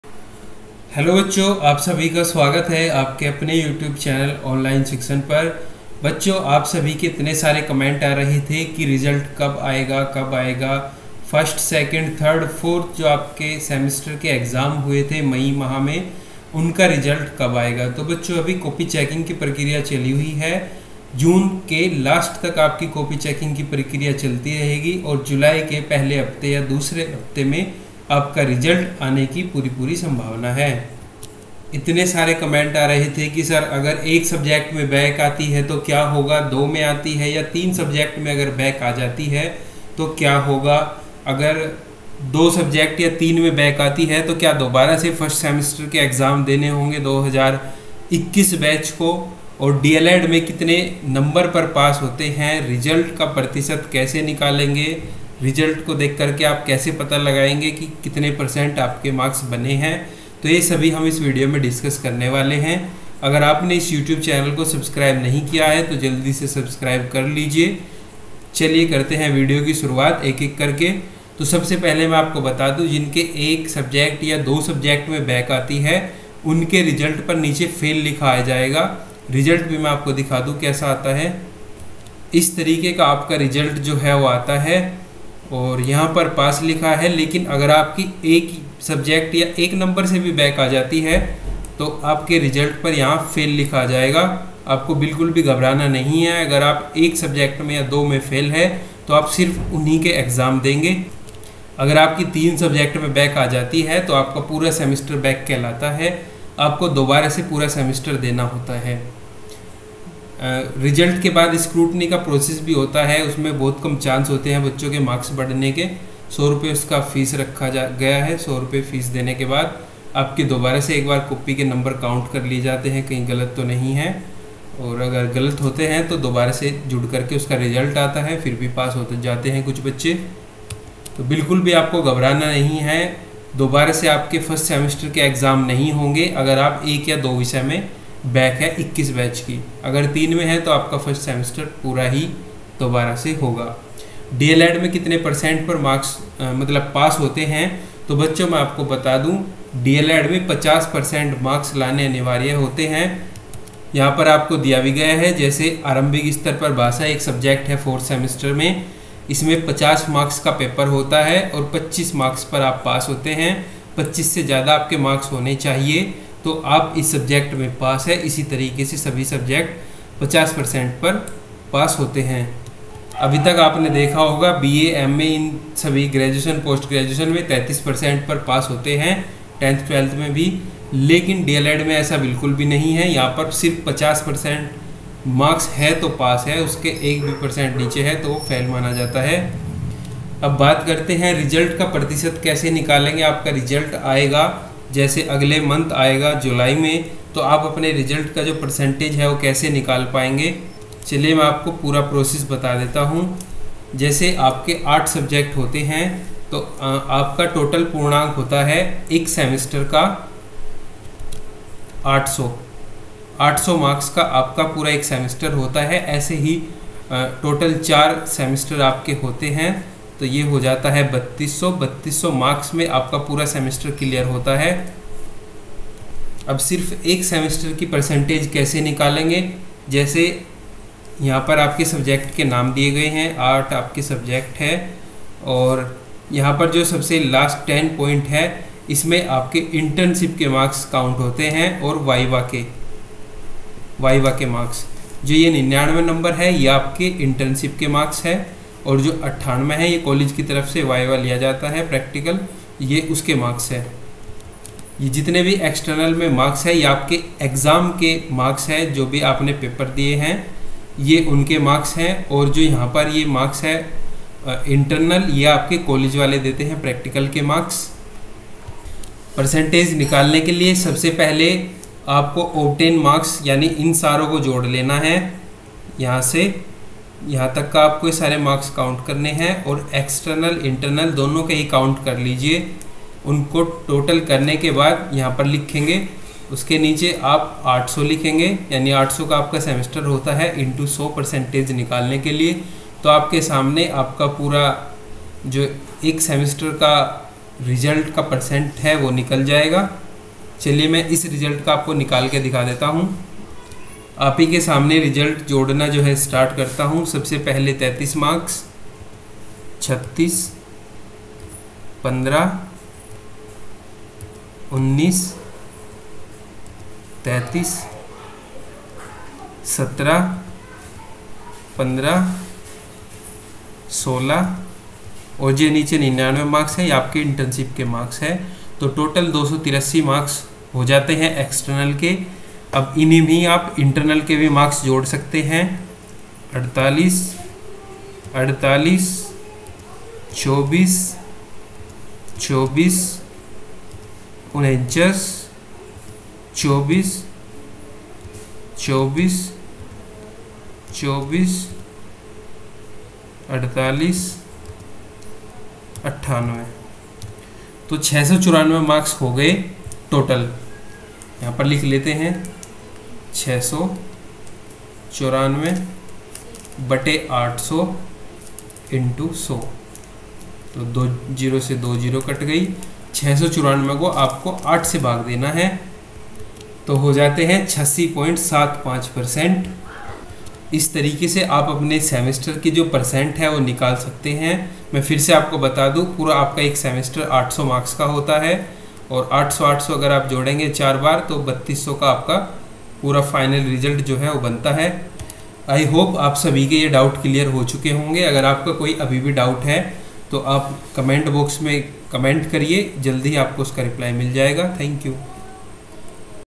Big Update,Deled result 2022 | डीएलएड रिजल्ट प्रतिशत कैसे निकाले | Deled मे कितने % पर Pass होते हैं
Hello Friends,

दोस्तों PDF के लिए टेलीग्राम पर भी हमें ज्वाइन करिए |

Note- अगर आप टेलीग्राम ग्रुप जॉइन नहीं कर पा रहे हैं तो आप टेलीग्राम पर जाकर online shikshan  सर्च कर सकते हैं